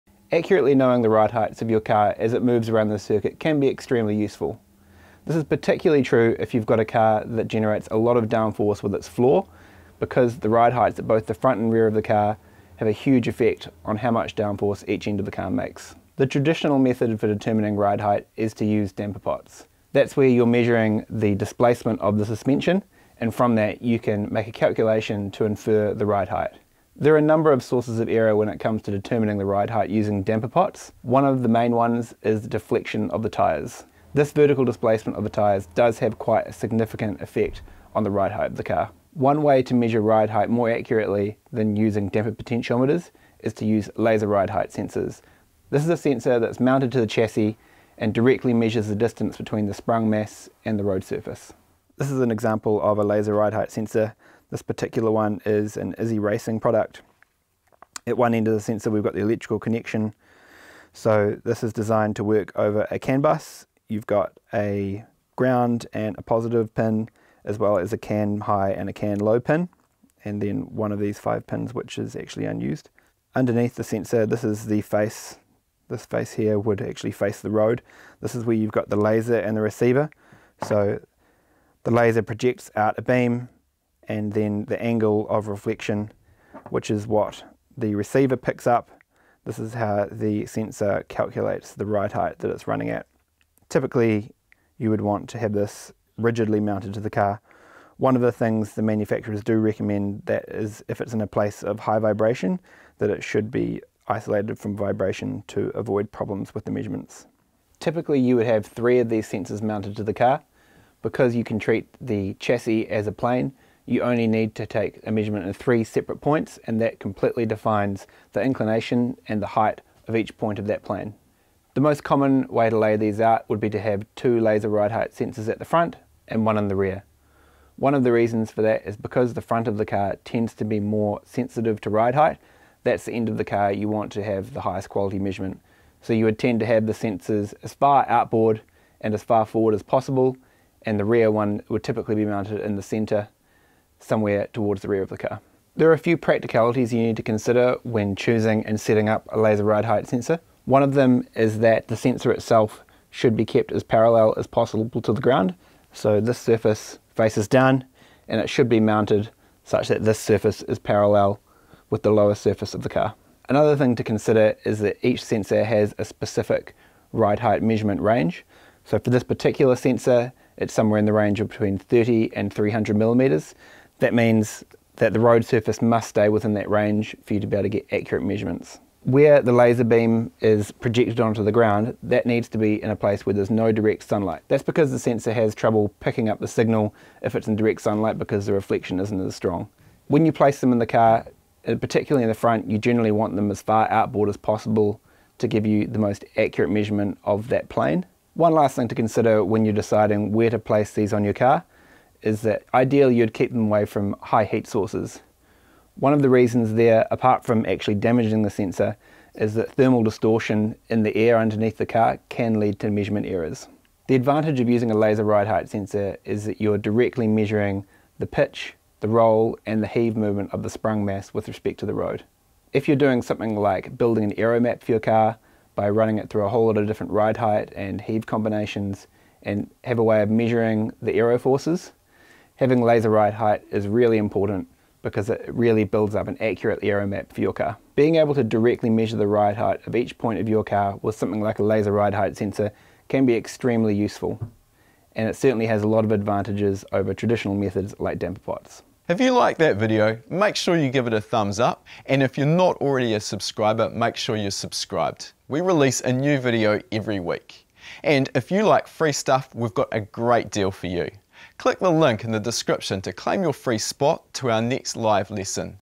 Why Use Laser RIDE HEIGHT Sensors? | Race Car Setup [TECHTALK]
Damper pots are one of the more common ways to measure what your ride height is doing as you make your way around a circuit or course, but they are susceptible to error particularly in relation to tyre dynamics, which is something a laser setup avoids as Tim from RaceCraft explains.

While superior, laser ride height sensors like those from Izze Racing don't come without their own complications either mainly when it comes to mounting to a solid surface with minimal vibrations (for a race car) and ensuring the laser hitting a shaded surface. Why? Hit the timestamp below and Tim will explain.

0:00 - Why Measure Ride Height?
0:16 - Damper Pots
0:26 - Damper Pot Sources of Error
0:40 - Laser Ride Height Sensor
1:37 - Mounting and Positioning
2:26 - Sensor Selection
2:58 - No Sunlight
3:18 - Thermal Distortion
3:34 - The Advantages of a Laser Ride Height Sensor
3:58 - Outro

Want to learn how to do your own motorsport wheel alignment? Start with a FREE live lesson